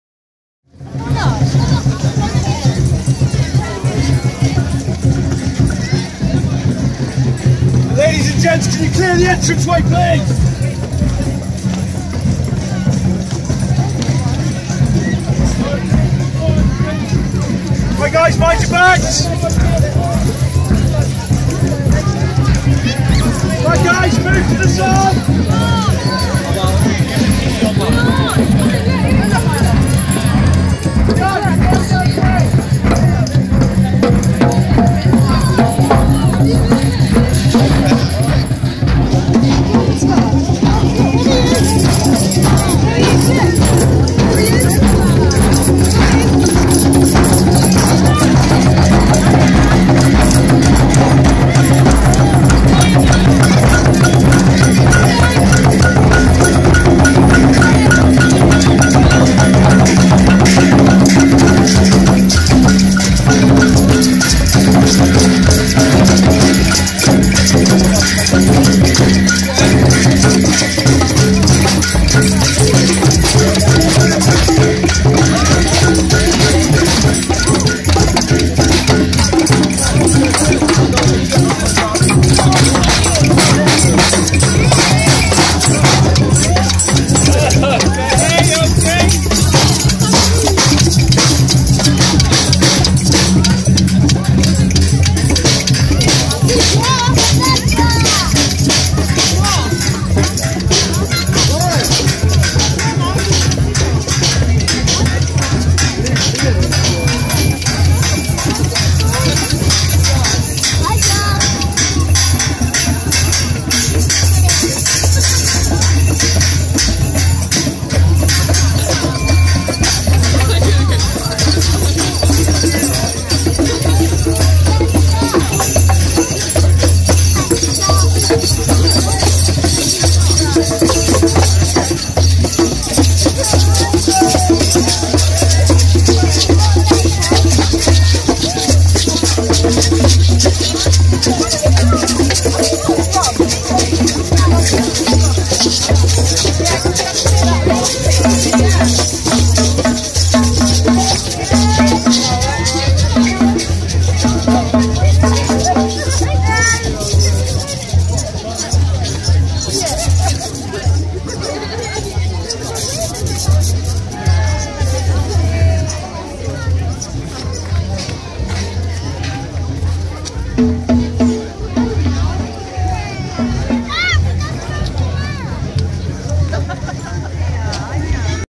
Respect Exeter procession 2015
Test of WiFi and topical issues. More soon. It could have been streamed live if the bandwidth allowed. As it was, recorded on phone then uploaded over wifi at home, but online about an hour after it happened. So live streaming getting closer.